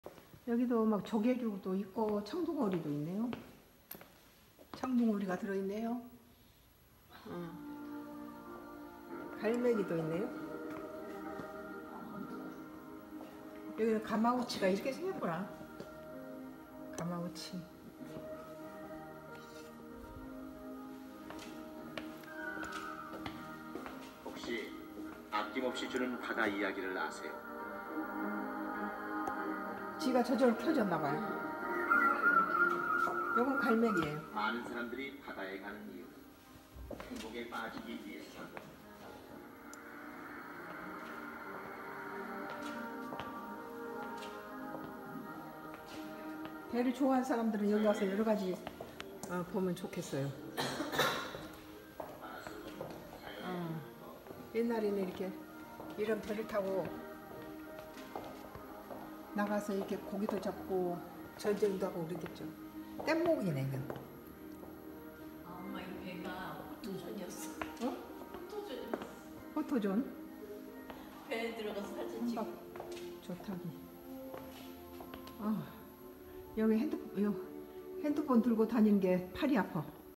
핸드폰 들고다니기 팔이 아파@sungodcross
우리모녀~~! 어촌 민속 박물관을 다녀오다 !

우리가족은~~! 일이 있어서 외출할때는~~~읍이나 면에 가는길에 ~~~
대여섯가지 일을 한꺼번에 해결하는 경향이 있다~~!

오늘! 면소재지 나가면서~~~주변에 있는 어촌 민속 박물관을 다녀오다~~~!
작품구상에도 도움이 되고~~ 어촌에 대하여 잘 몰랐는데~~~!
여러가지 배의 구조나 어구에 대하여 바닷생물에 대하여~~~많은 도움이 되었다~~!@sungodcross